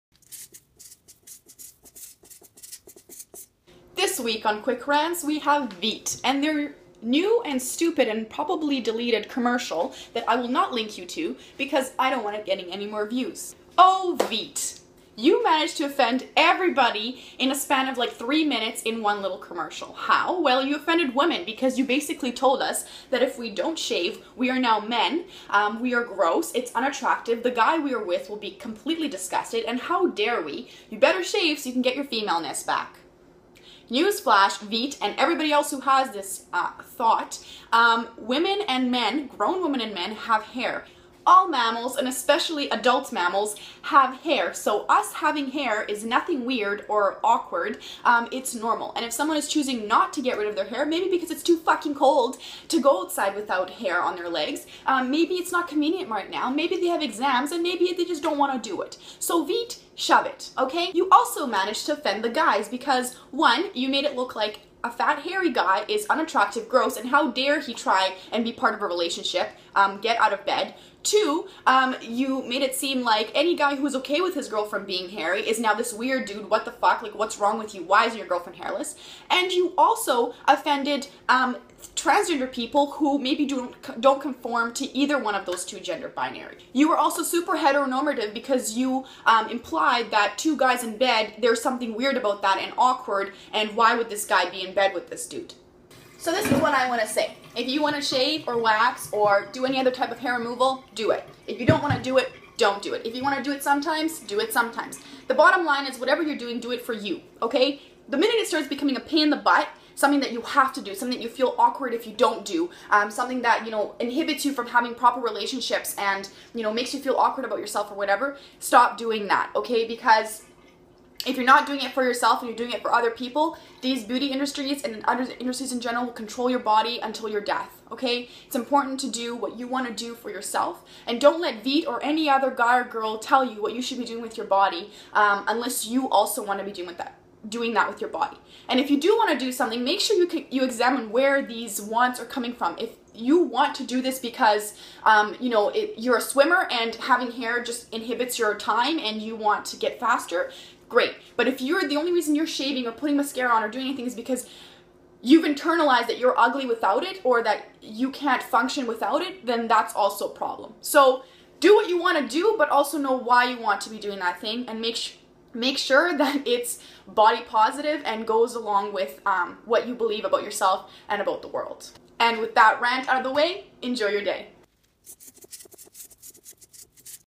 Quick Rants: Veet's Stupid Commercial
I rant about BS I see and call people and companies out on it.

Also, Veet, your "apology" was crap. You can't insult me and then play it off like I (and millions of others) "didn't get" your humour. As a result, I'm still